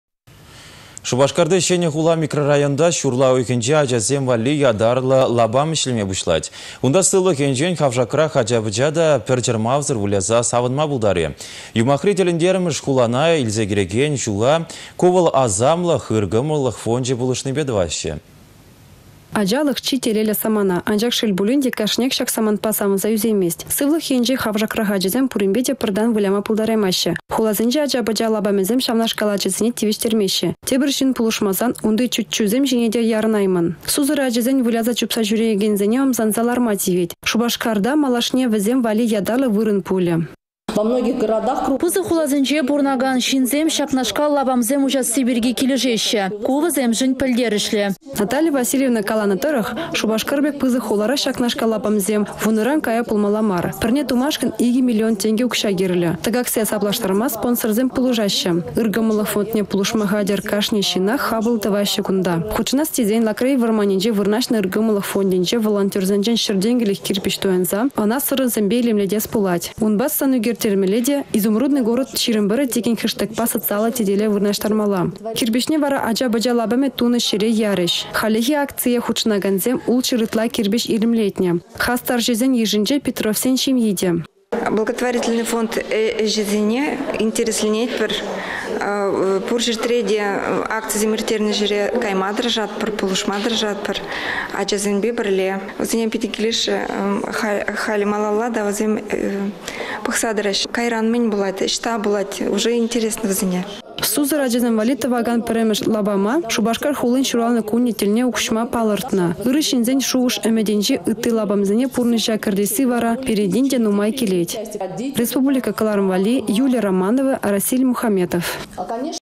«Изумрудран тунă хулана илсе кĕрекен çул »
Шупашкарти Çĕнĕ хула микрорайонта çурла уйăхĕнче ачасем валли ятарлă лапам ĕçлеме пуçлать. Унта сывлăх енчен хавшакрах ача-пăча та пĕр чăрмавсăр выляса савăнма пултарĕ. Юмахри тĕлĕнтермĕш хулана илсе кĕрекен çула «Ку вăл асамлăх» ыр кăмăллăх фончĕ пулăшнипе тăваççĕ.